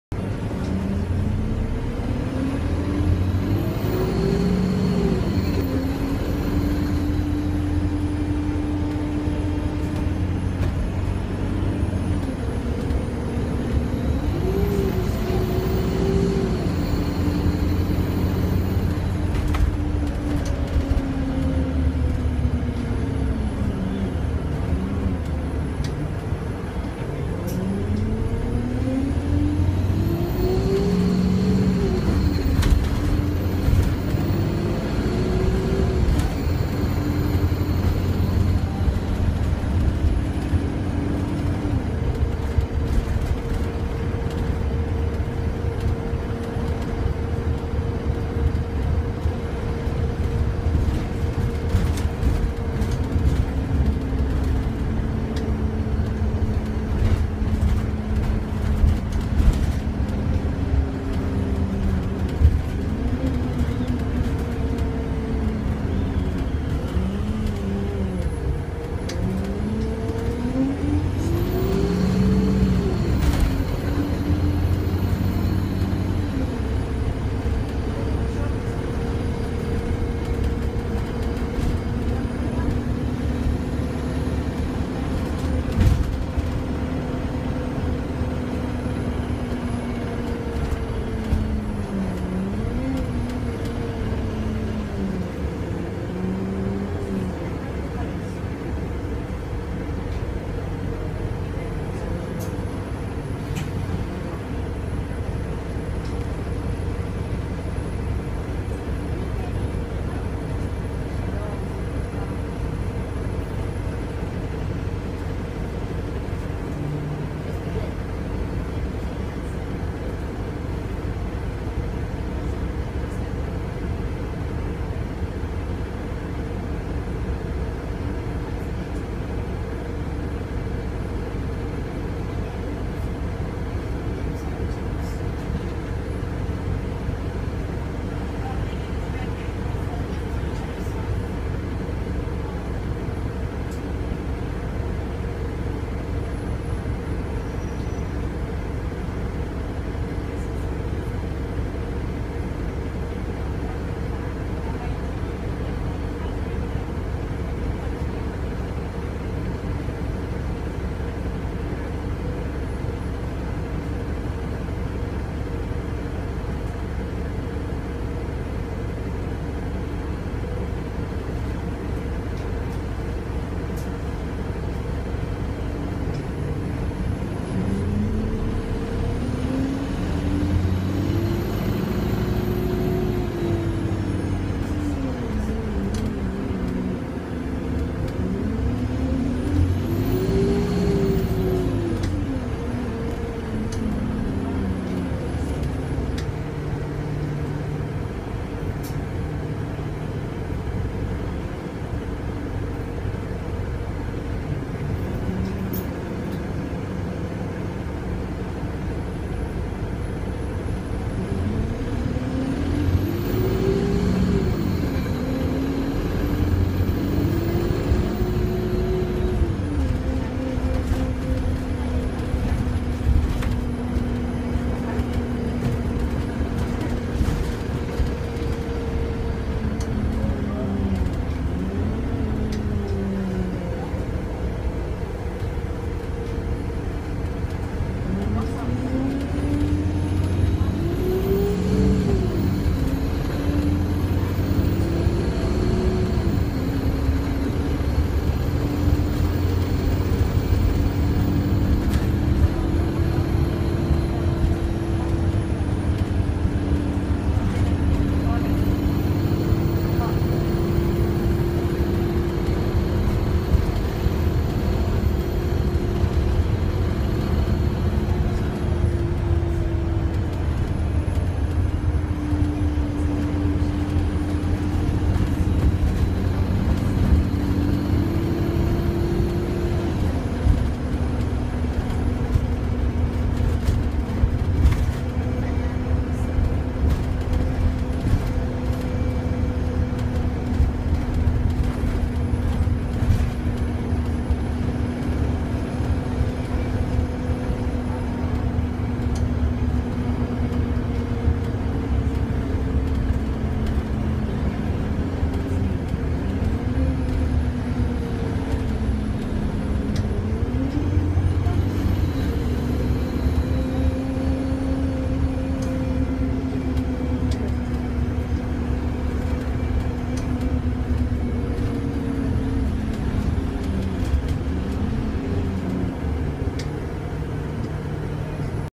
*BRAND NEW* Route 236 Moffat & Williamson SN66 WHR Alexander Dennis Enviro 200 MMC Voith
On its first day in service in Dundee on the 236 to Claverhouse.